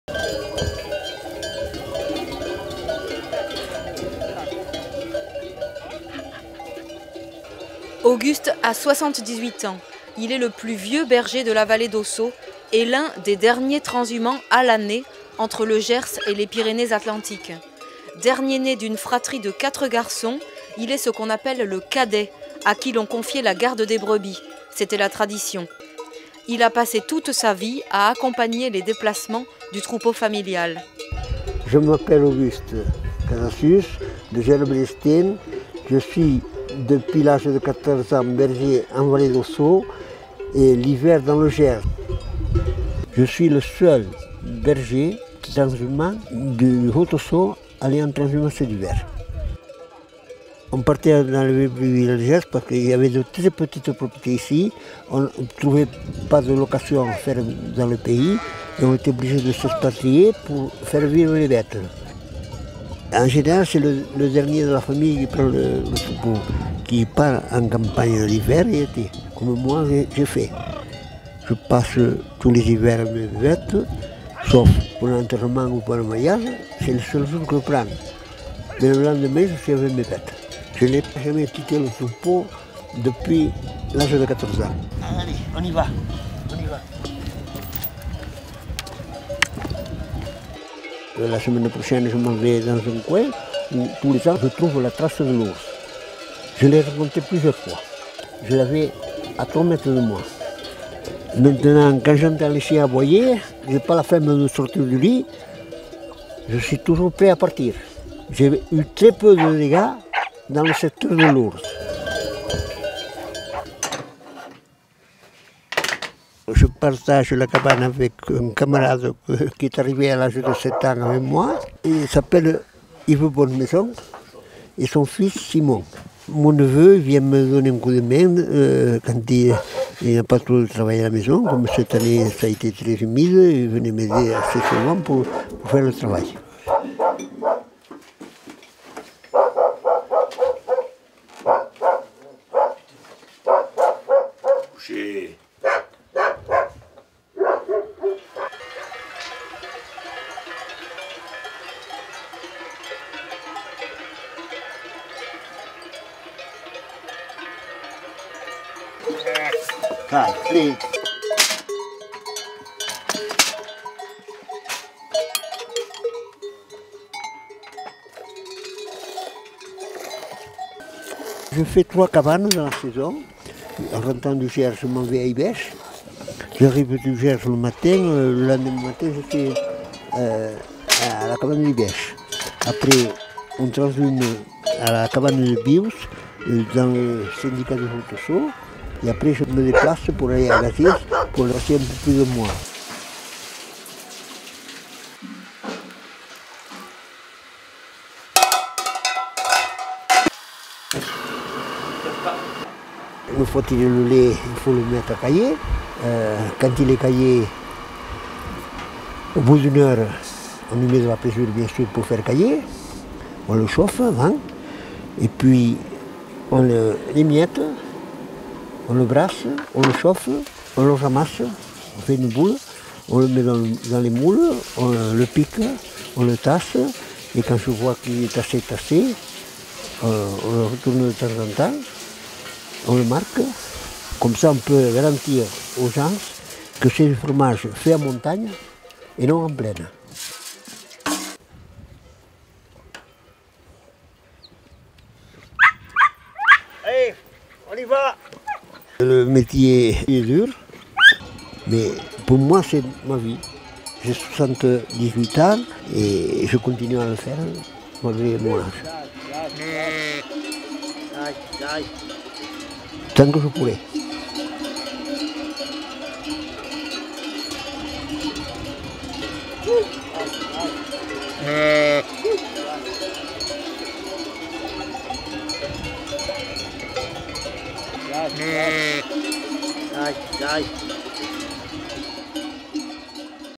Auguste, berger transhumant.mp4
Auguste était le dernier berger transhumant à l'année en vallée d'Ossau, dans les Pyrénées-Atlantiques. Il était le cadet de famille, celui à qui l'on confiait la garde du troupeau.